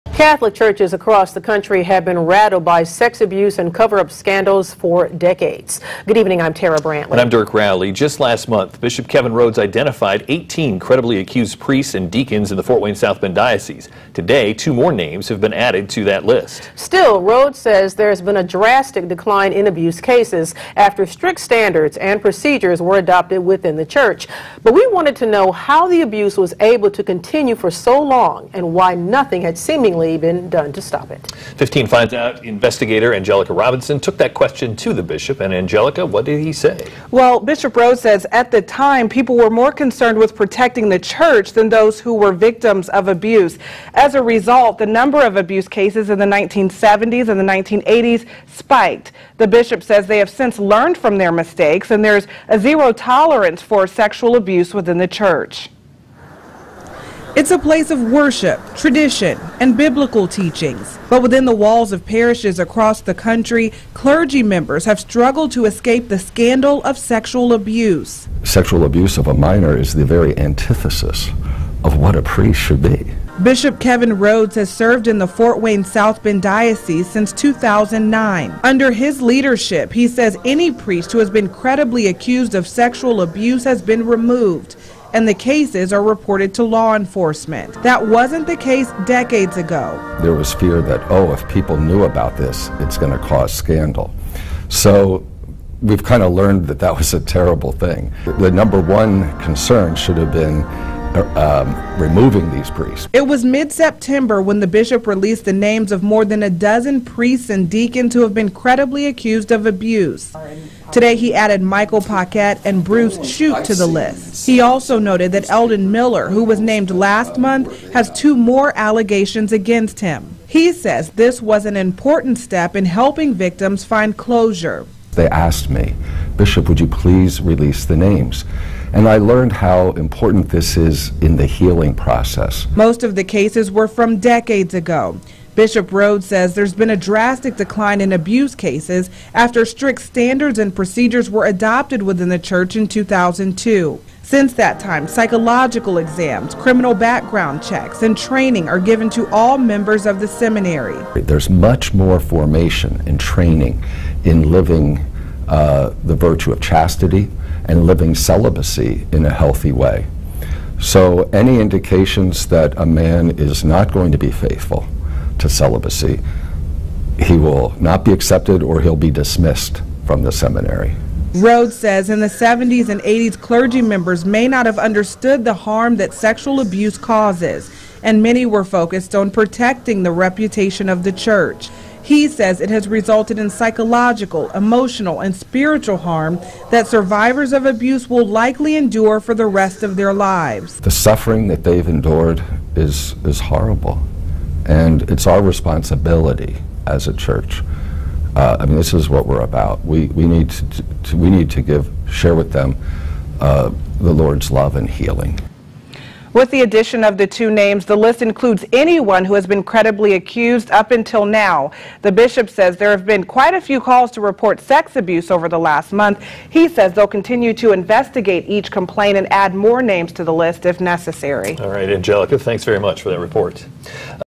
Fort Wayne Bishop names two more priests accused of sexual abuse
WANE 15'S Angelica Robinson reports.